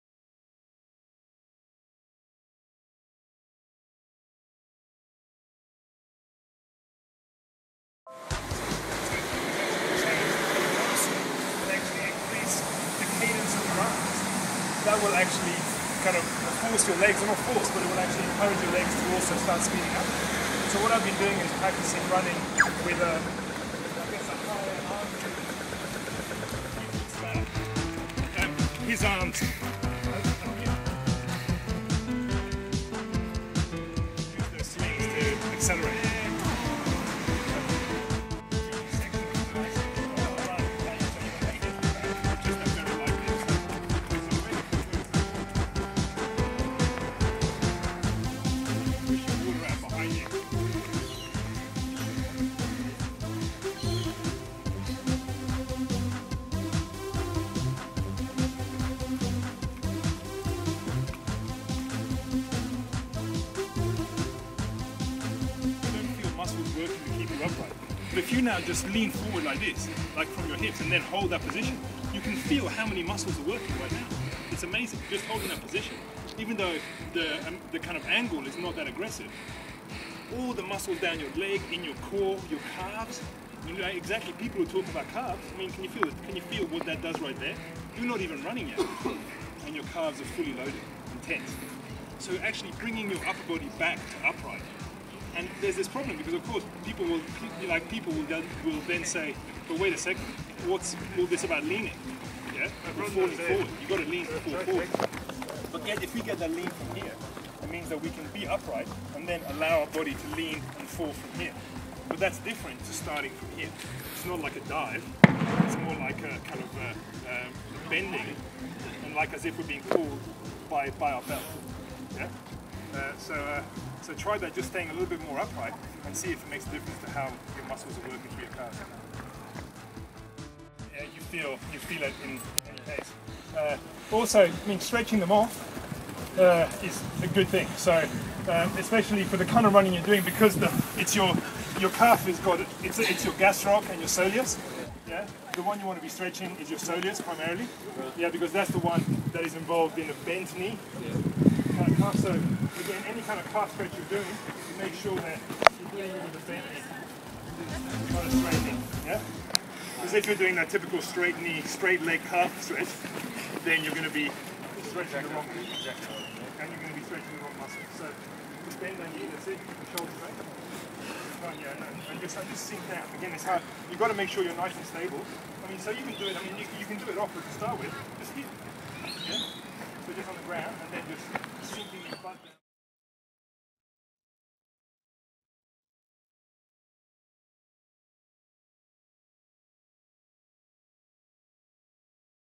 Barefoot Running Clinic 17th June 2011 Adventure Megastore.mp4
This is the first of our lunchtime barefoot running clinic's. Today's session was taken by none other than Max Delay of Barefoot Inc (the premier minimalist shoe importer and distributor in Australian with Vibram FiveFingers, Inov-8 and Zem Gear).

It had been terrible weather during the week so only a few brave soles joined us, but all had a great time on our run - and learnt some great insights into natural/pose running.